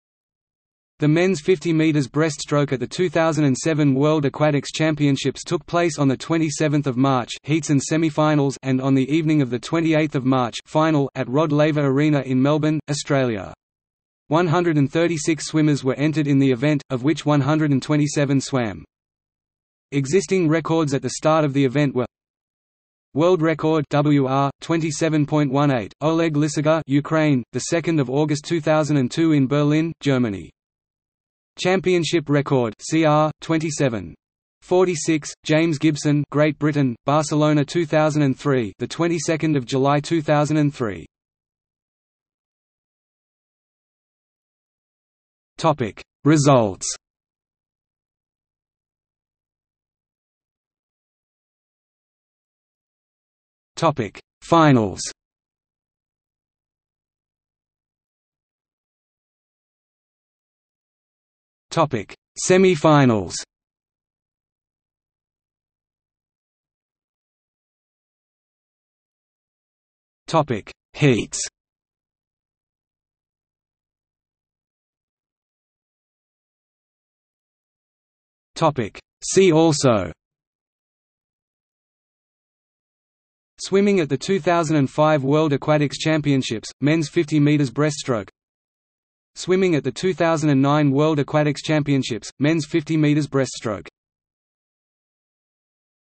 Swimming at the 2007 World Aquatics Championships – Men's 50 metre breaststroke | Wikipedia au ...
00:00:42 1 Results
00:00:50 1.1 Finals
00:00:58 1.2 Semifinals
00:01:06 2 Heats
00:01:15 3 See also

- Socrates

SUMMARY
The men's 50 metre breaststroke at the 2007 World Aquatics Championships took place on 27 March (heats and semifinals) and on the evening of 28 March (final) at Rod Laver Arena in Melbourne, Australia. 136 swimmers were entered in the event, of which 127 swam.
Existing records at the start of the event were:

World record (WR): 27.18, Oleg Lisogor (Ukraine), 2 August 2002 in Berlin, Germany.
Championship record (CR): 27.46, James Gibson (Great Britain), Barcelona 2003 (22 July 2003)